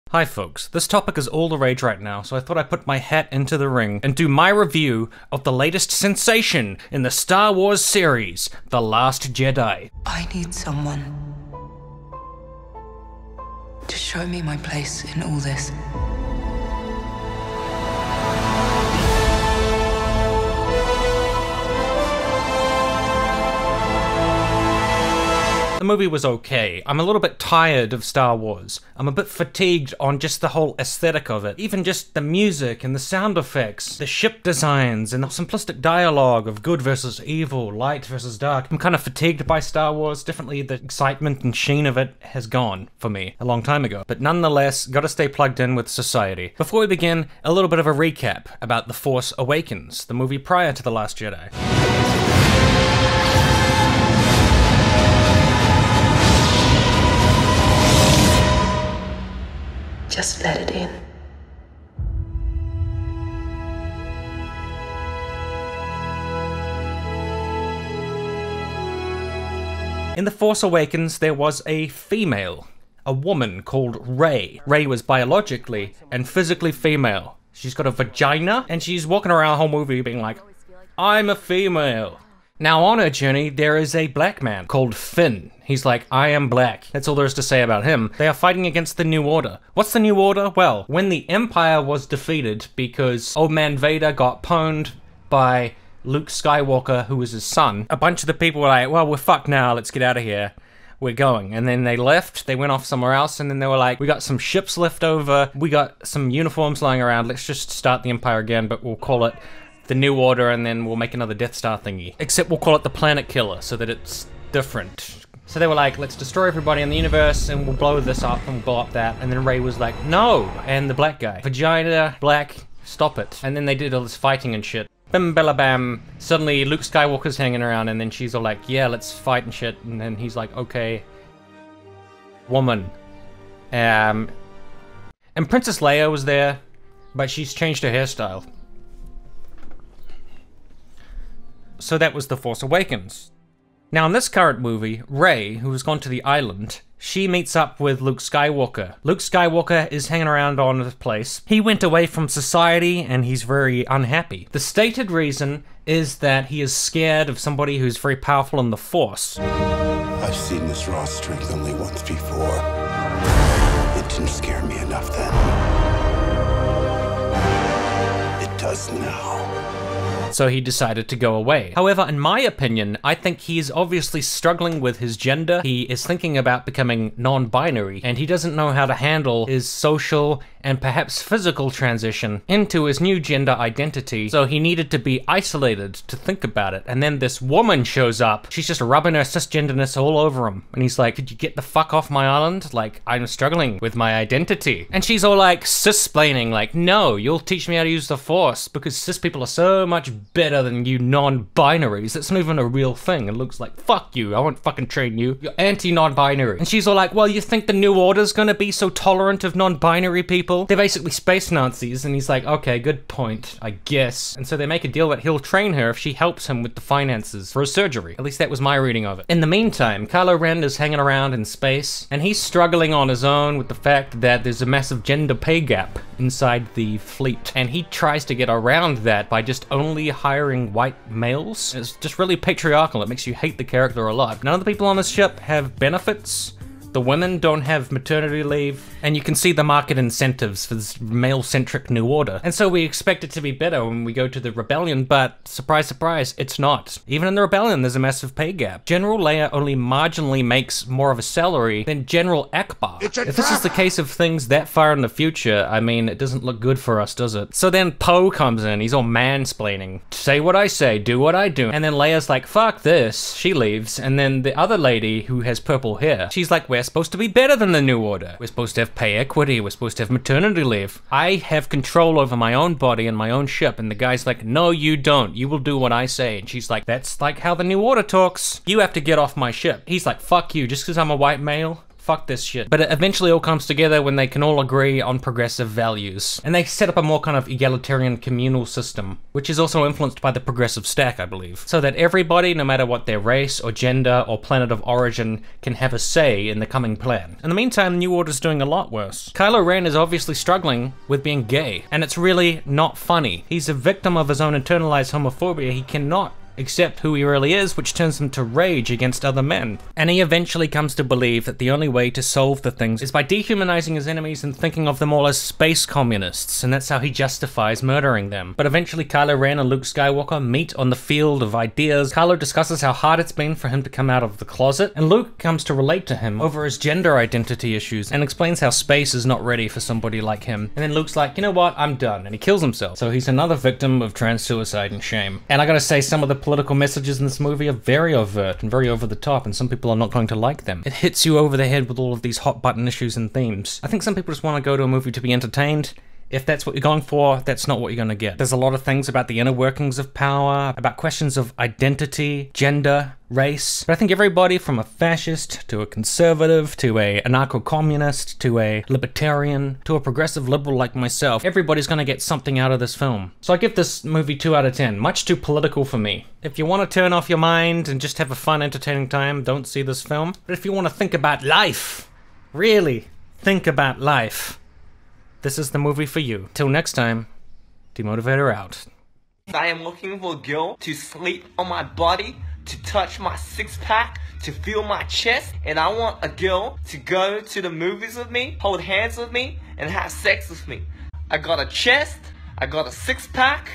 The Last Jedi: A Post Structural Proto-Marxist Analysis
Karl Marx wrote: "Capital is dead labor, which, vampire-like, lives only by sucking living labor, and lives the more, the more labor it sucks".  No other words are more relevant when analysing Star Wars: The Last Jedi, and it is through this Marxist lens that I desconstruct this particular manifestation of bourgeois meta-narration, otherwise known as Star Wars.

*SPOILER WARNING*

Will the workers finally seize the means of starkiller base production?  And will the gender pay gap finally be closed between Princess Leia and Admiral Akbar?

If you don't want to know, wait to see the film.  I would recommend "The Origin of the Family, Private Property and the State" by Richard Engels before doing so, in order to have a strong theoretical framework from which to apply your dialetical material lens to the work.

Tell me how you relate this work to your own school of Marxism in the comments.  :-)

Cheers,